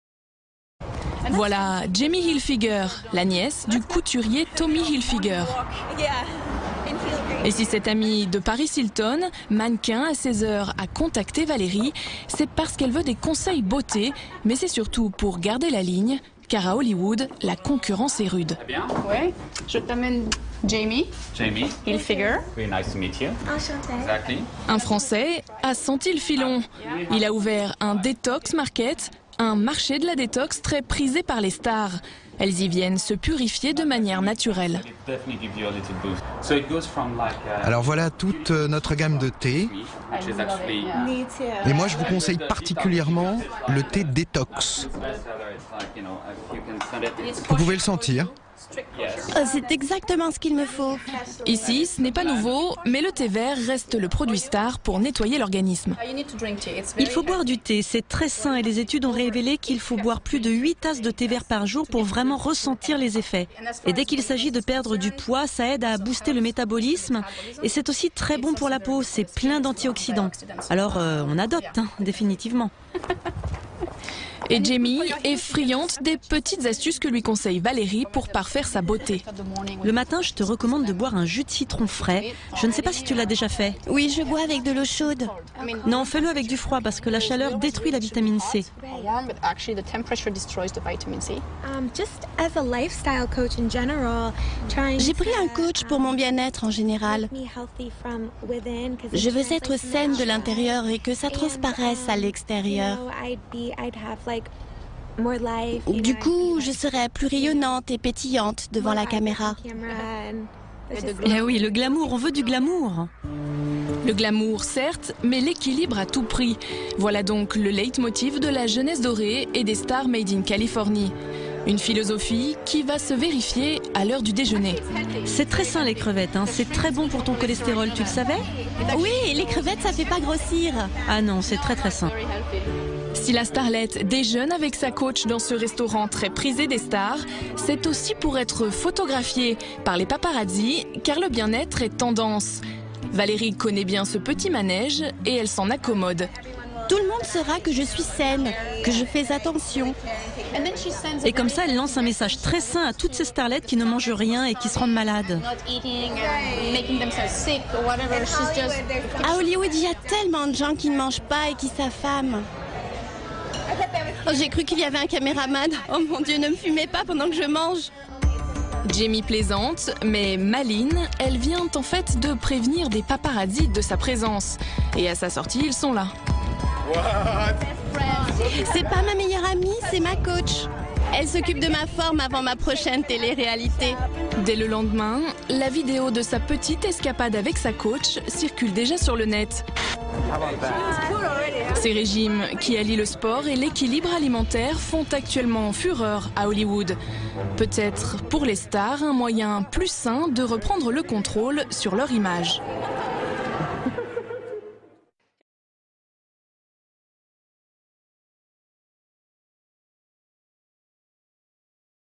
Jaimie Hilfiger on M6 TV France Beauty Advice
Supermodel Jaimie Hilfiger and healthy living coach Valerie Orsoni (lebootcamp.com) on M6 France.